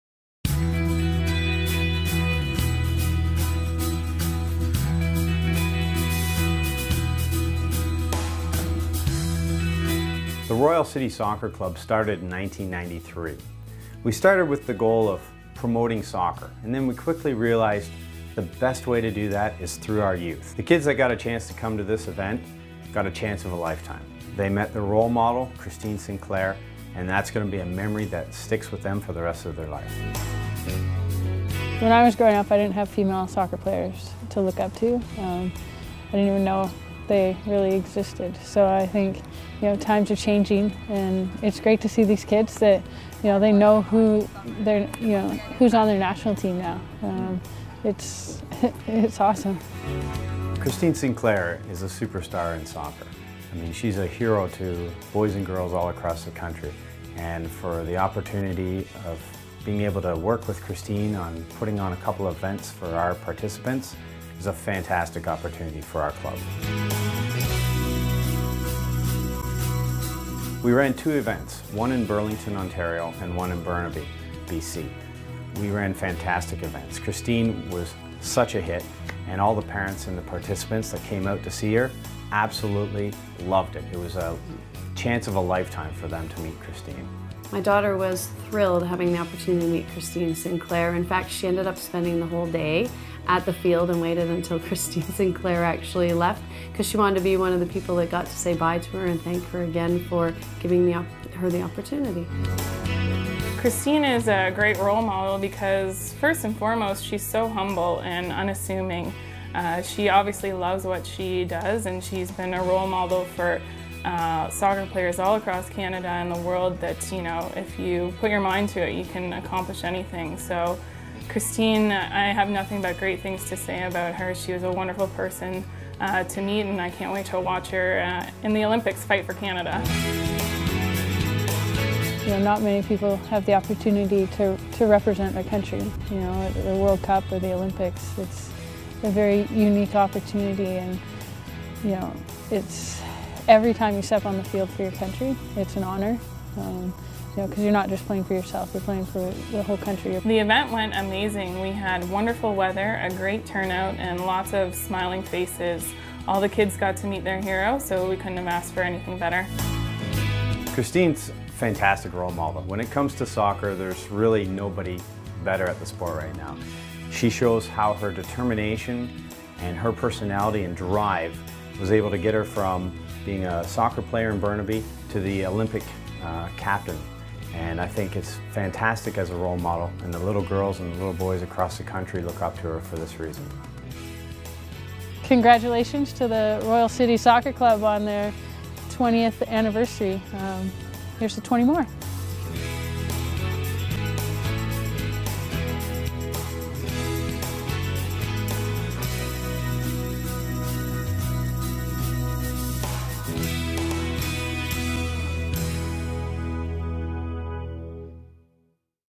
Canadian Soccer Legend Christine Sinclair visits Royal City Soccer Club
Christine Sinclair, Captain of the Women's National Team and one of the greatest soccer players of all-time visited Royal City Soccer Club to share a little bit of her magic!